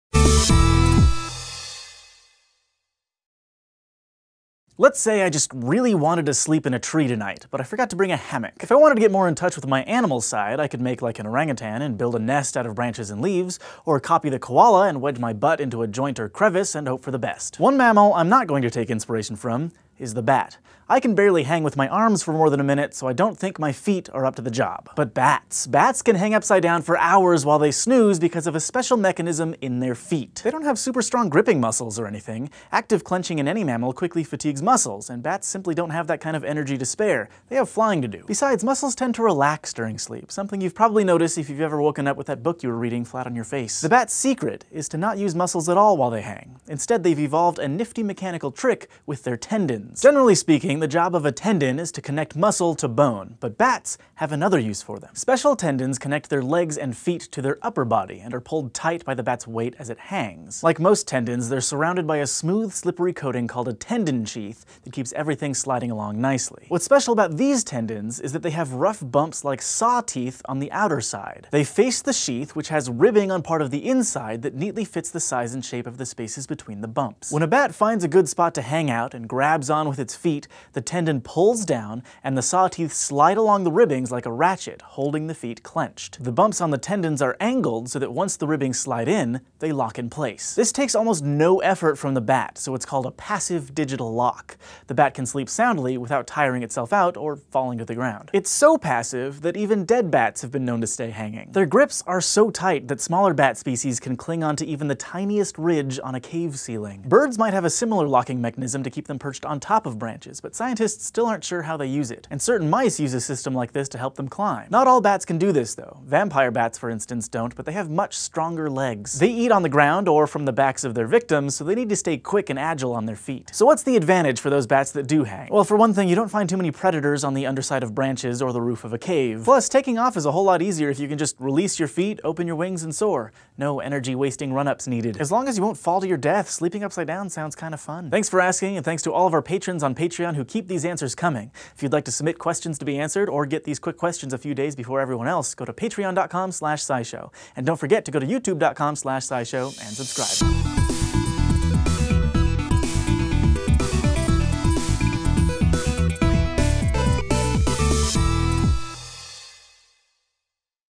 Why Don't Sleeping Bats Fall Down?
Bats sleep upside down, so how come they don’t fall? Turns out that they’ve got some unusual legs.

Vampire bats don’t have a “passive digital lock” (could not access full article but this was stated in abstract)